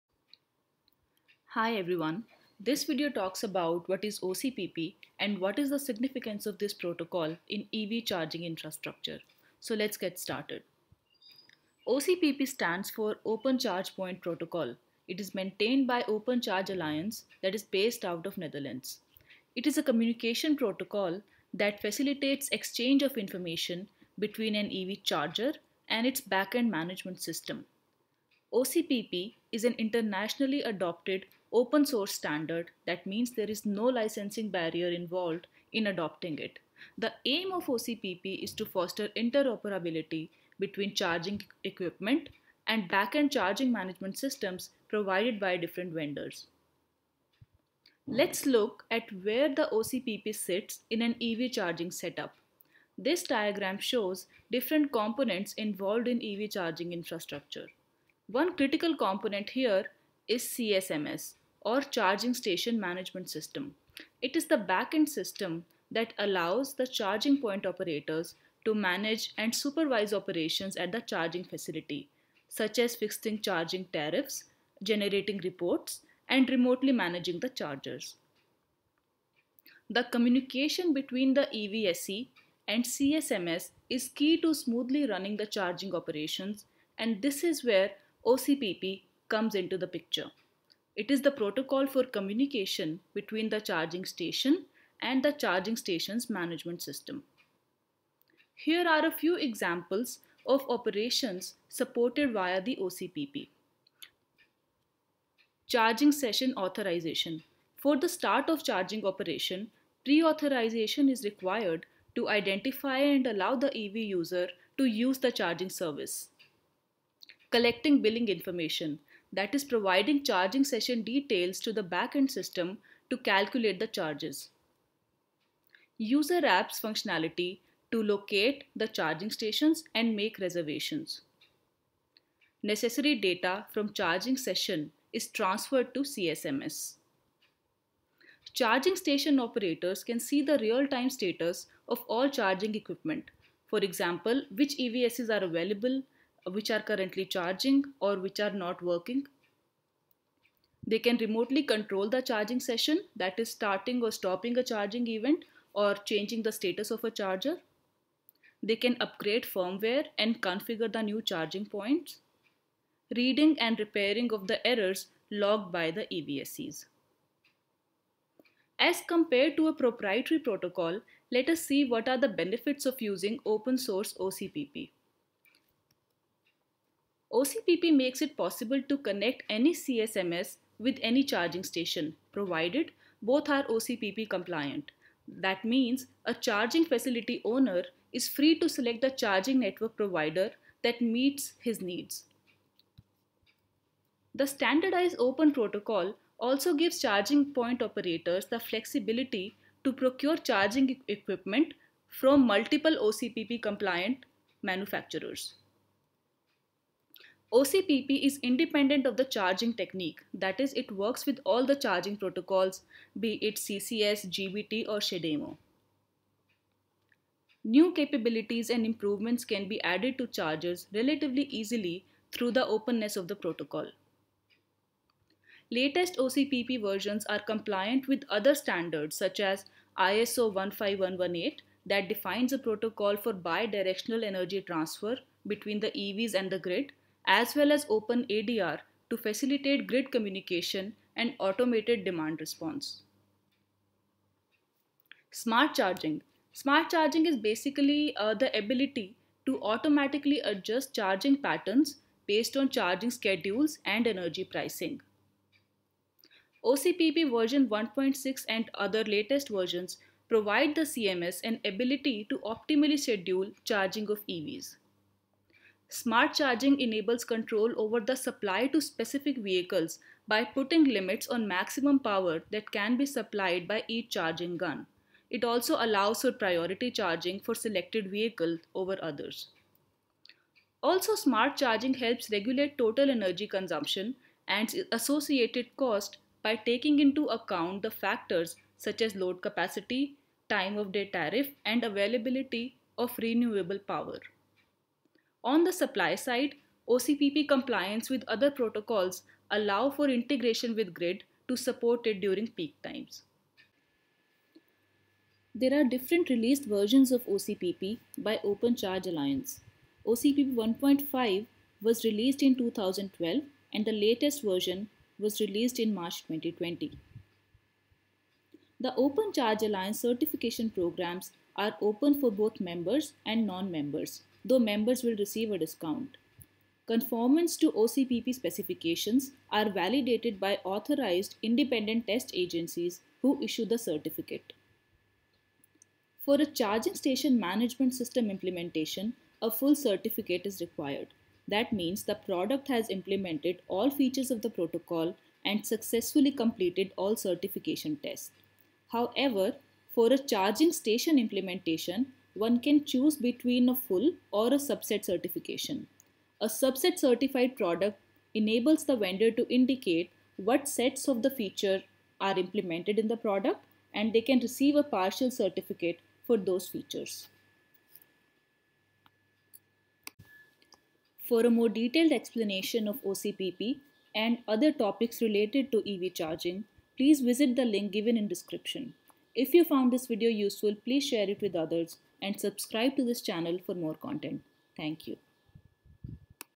What is OCPP? What is OCPP's relevance to Electric Vehicle Charging?
OCPP helps the EVSE (Electric Vehicle Supply Equipment) and CSMS (Charging station management system) to communicate with each other through its open platform. This video describes the basics of OCPP and its significance in an EV Charging Setup.

Transcript: This video talks about What is OCPP and what is the significance of this protocol in Electric Vehicle Charging Infrastructure.

OCPP stands for Open Charge Point Protocol, maintained by Open Charge Alliance based out of Netherlands. It is a communication protocol that facilitates the exchange of information between an EV charger and its backend management system. OCPP is an internationally adopted Open source standard - that means there is no licensing barrier involved in adopting it. The aim of OCPP is to foster interoperability between charging equipment and backend charging management systems provided by different vendors.

Let’s look at where the OCPP sits in an EV charging setup:
This diagram shows the different components involved in EV charging infra. One critical component here is CSMS or Charing stations management system. It is the back-end system that allows the charging point operators to manage and supervise operations at the charging facility, such as fixing charging tariffs, generating reports and remotely manage the chargers. Communication between EVSE and CSMS is key to smoothly running the charging operations and this is where OCPP comes into picture. It is the protocol for communication between the Charging station and Charging stations management system.

Here are a few examples of operations supported via the OCPP:
For the start of charging operation, pre-authorisation is required to identify and allow the EV user to use the service. Providing the charging session details to backend system, to calculate the charges
User apps functionality to locate the charging stations and make reservations. Necessary data from charging session is transferred to CSMS. Charging stations operators can the see real-time status of all charging equipment - e.g. which EVSEs are available, currently charging or suspended. Remotely control the charging session - stop or start a charging event or change the status of a charger. Upgrade firmware and configure the charging points. Read and repair errors logged by the EVSEs.

As compared to a proprietary protocol, what are the benefits of OCPP? OCPP makes it possible to connect any CSMS with any charging station, (provided both are OCPP compliant). A charging facility owner is free to select the charging network provider that meets his needs.
The standardized open protocol gives charge point operators the flexibility to procure charging equipment from multiple OCPP compliant manufacturers. OCPP is independent of the charging technique, it works with all charging protocols— CHAdeMO, CCS or GB/T. New capabilities and improvements can be added to the chargers relatively easily through the openness of the protocol.
Latest OCPP versions are compliant with other standards such as ISO 15118 that defines a protocol for bi-directional energy transfer between EVs and the Grid, as well as OpenADR to facilitate grid communication and automated demand response.

Smart Charging - refers to the ability to automatically adjust charging patterns, based on charging schedules and energy pricing. OCPP (version 1.6 onwards) provides the CSMS an ability to optimally schedule charging of EVs. Smart Charging enables control over supply to specific vehicles by putting limits on maximum power that can be supplied by each charging gun. Also allows priority charging for selected vehicles over others. Also, smart charging helps regulate total energy consumption and its associated cost by taking into account the factors such as load capacity, Time of Day tariff and availability of renewable power. On supply side, OCPP compliance with other protocols allows for integration with Grid to support it during peak demand time.

These are the released versions of OCPP by Open Charge Alliance: OCPP 1.5 released in 2012, OCPP 2.0. 1 (J) released in March 2020

The certification programs are open for members and non-members. Conformance to OCPP specifications is validated by authorised independent test agencies to issue the certificate. For a Charging Station Management System implementation, a full certificate is required, that means the product has implemented all features of the protocol and has successfully completed all certification tests. For a charging station implementation, one can choose either a full or a subset certificate. A Subset Certified product enables the vendor to indicate what sets of features of OCPP 1.6 are implemented in the product and to receive a partial certificate for those specific features.